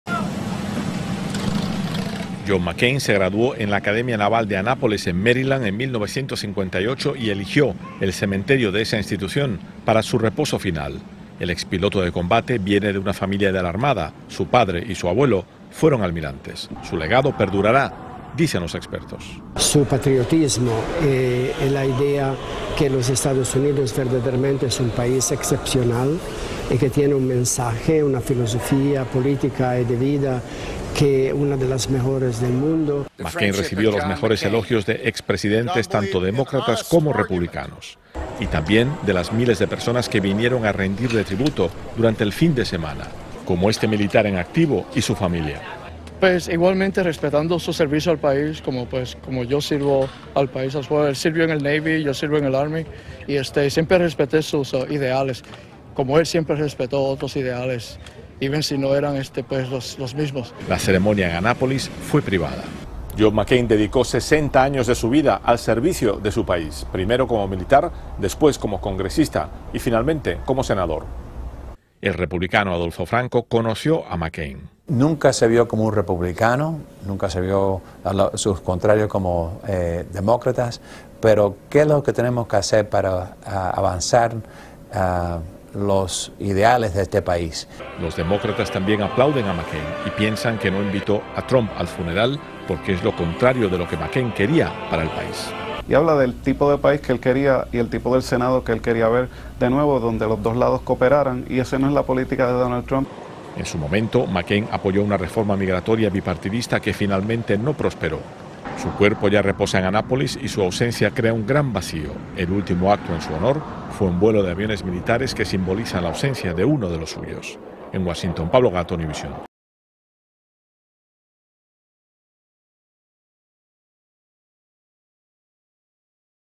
Entierran al senador John McCain en una ceremonia privada en el cementerio de la Academia Naval
La ceremonia en Maryland, a la que únicamente acudieron sus familiares y amigos cercanos, fue planeada por McCain durante los últimos meses. El senador decidió ser enterrado en ese lugar en vez del Cementerio Nacional de Arlington, un lugar generalmente reservado para veteranos y personalidades destacadas.